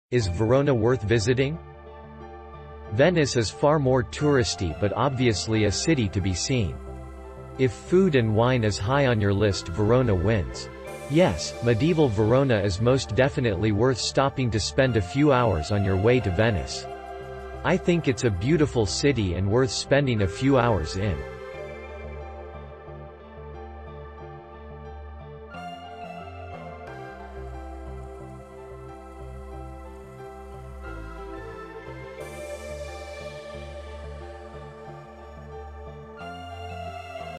Is Verona worth visiting?
Venice is far more touristy but obviously a city to be seen. If food and wine is high on your list Verona wins. Yes, medieval Verona is most definitely worth stopping to spend a few hours on your way to Venice. ... I think it's a beautiful city and worth spending a few hours in.

Reference:
Verona for the afternoon? - Verona Message Board - TripAdvisor